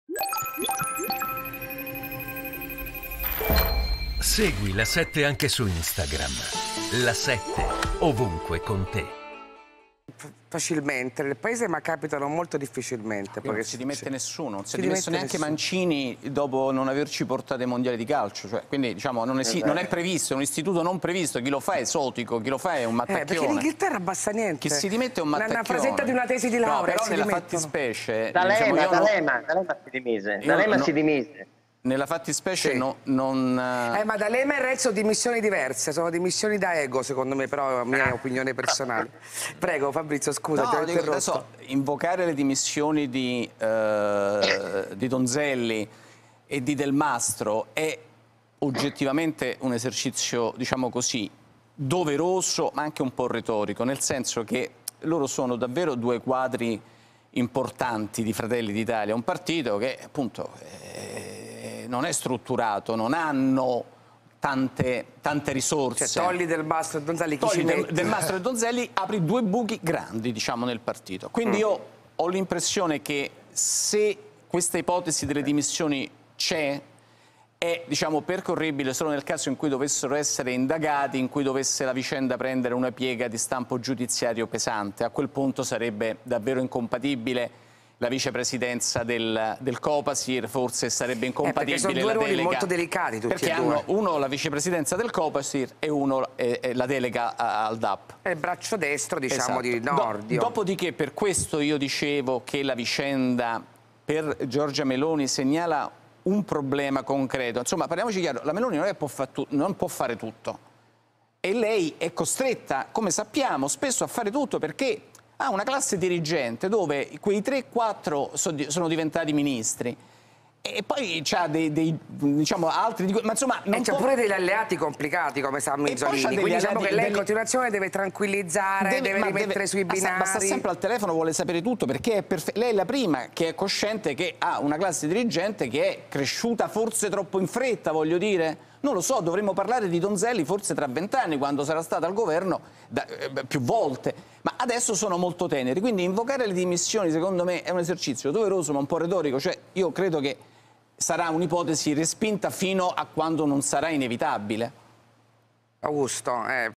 Roncone: "Dimissioni Donzelli e Delmastro? Non si è dimesso nemmeno Mancini..."
Fabrizio Roncone non crede alle possibili dimissioni di Donzelli e Delmastro dopo le divulgazione delle carte 'sensibili' su Cospito